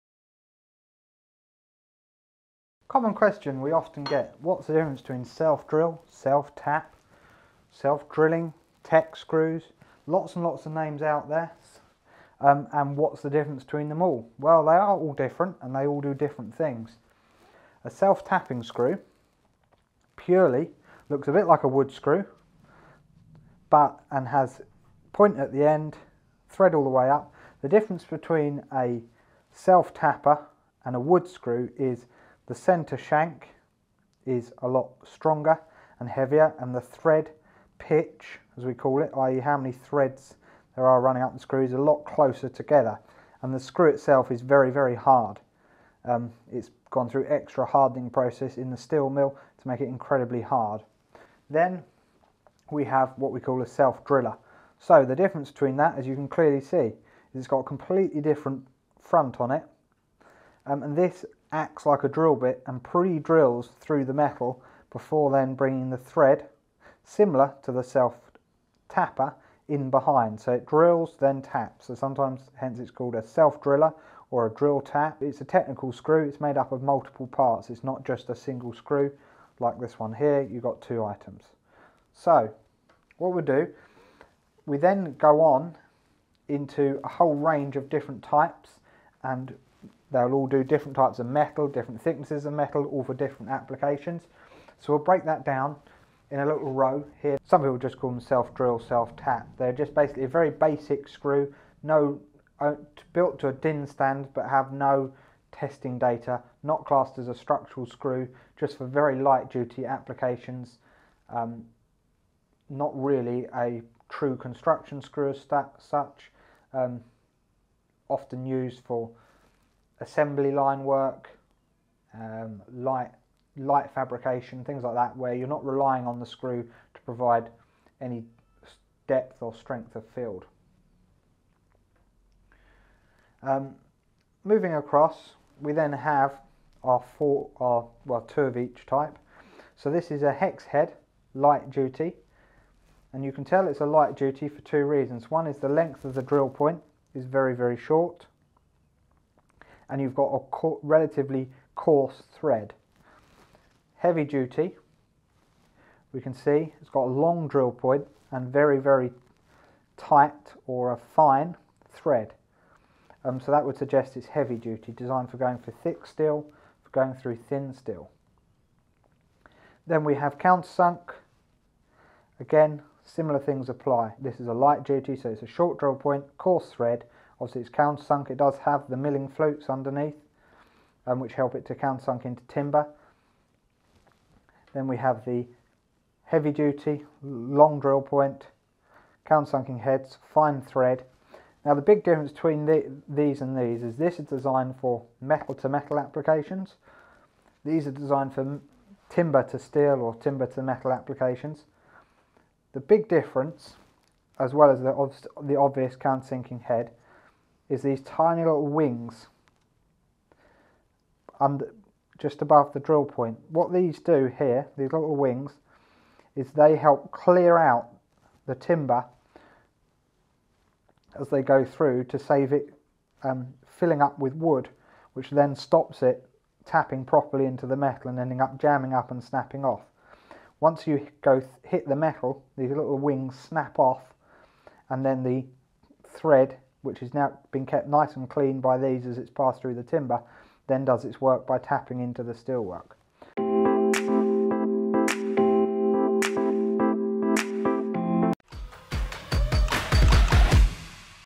The difference between self tapping, self drilling and CSK screws.
Walk through the basic facts and theory of what the difference between self-tap, self-dril and countersunk screws.